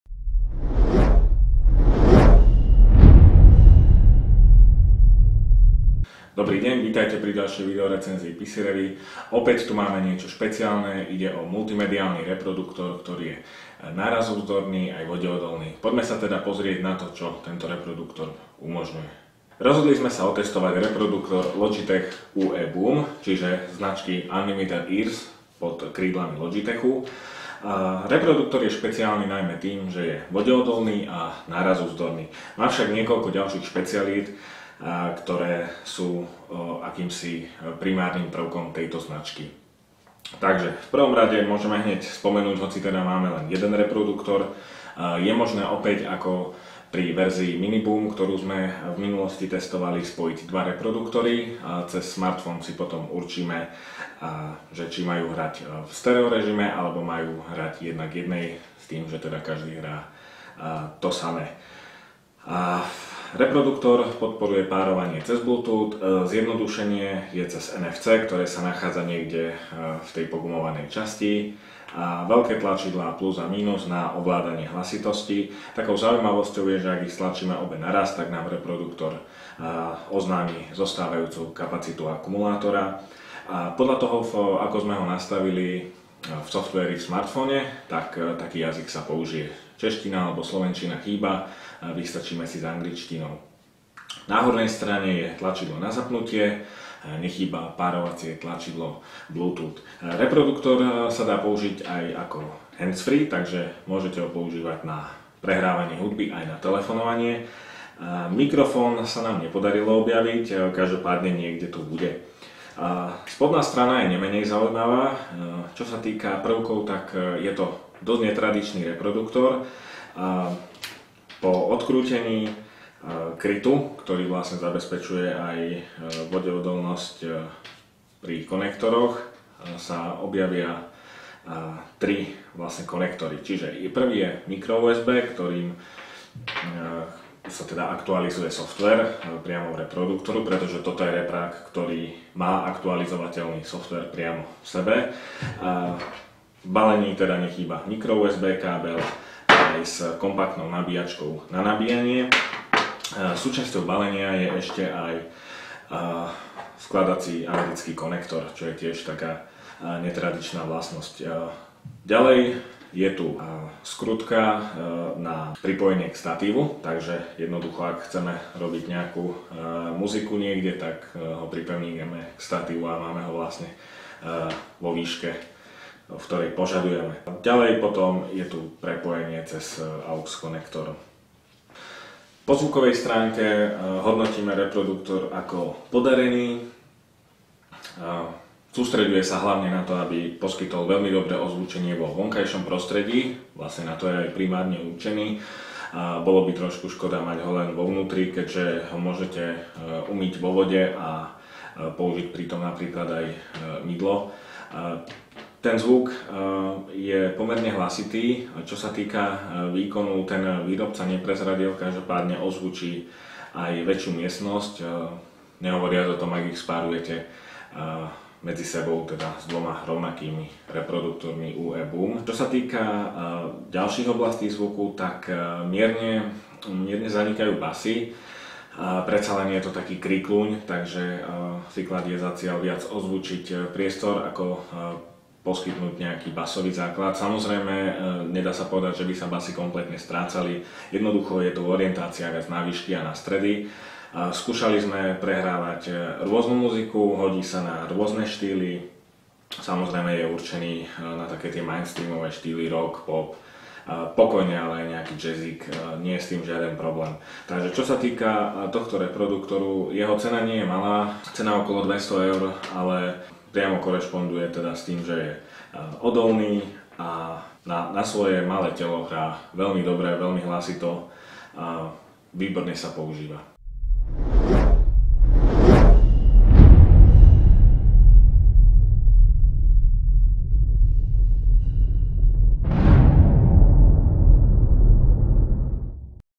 Reproduktor Logitech UE BOOM
Predstavujeme prenosný a vode odolný reproduktor Logitech UE BOOM. Hrá vynikajúco a výkonne, pritom dlho vydrží na batériu